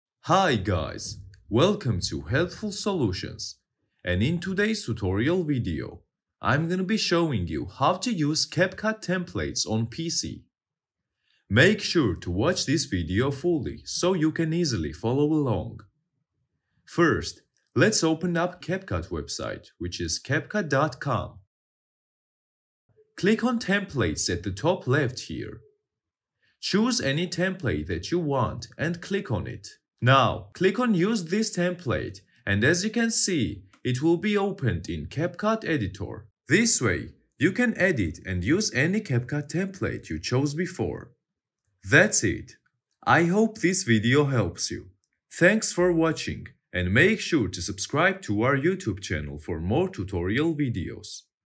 How To Use CapCut Templates On PC (How To Get CapCut Templates On PC)
How To Use CapCut Templates On PC (How To Get CapCut Templates On PC)s. In this video tutorial I will show how to use Capcut templates on PC.